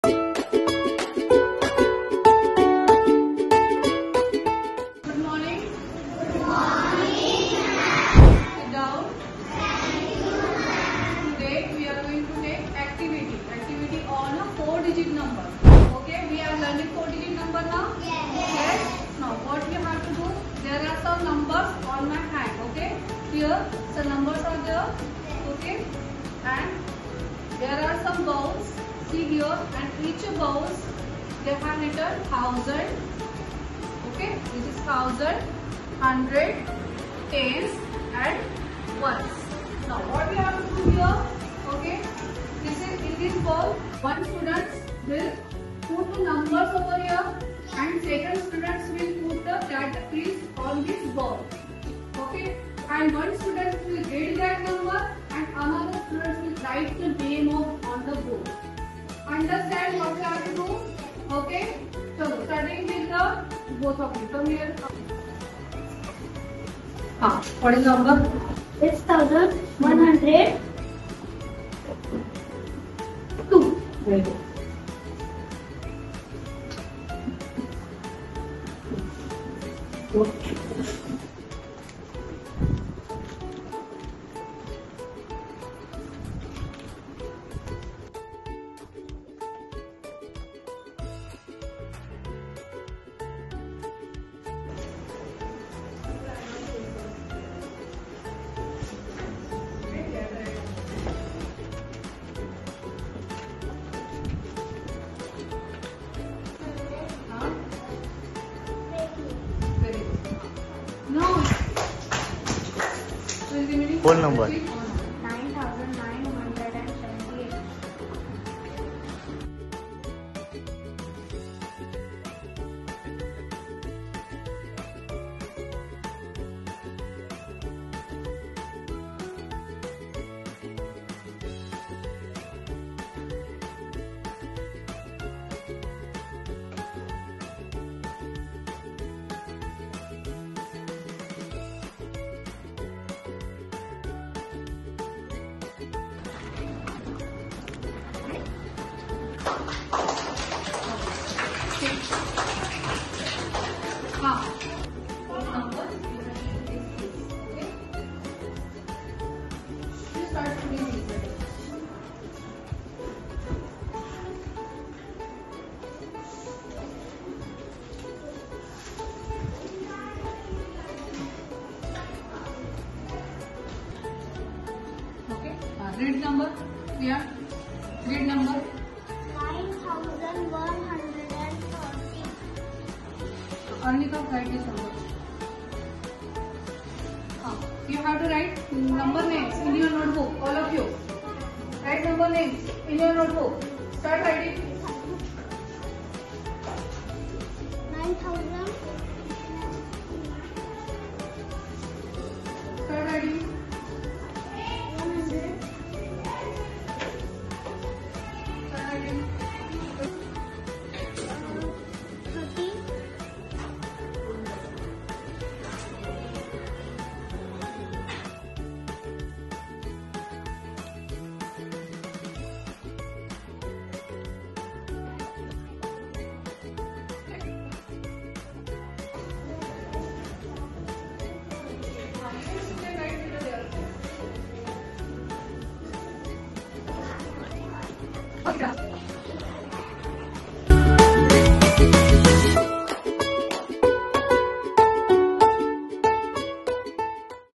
“Fun Math Activities for Kids | Learn While You Play!” Class 3 📚
Struggling with math? Try these engaging math activities designed to make concepts clearer and more enjoyable! Great for students of all ages to practice arithmetic, logic, problem-solving, and more. Perfect for school or revision at home.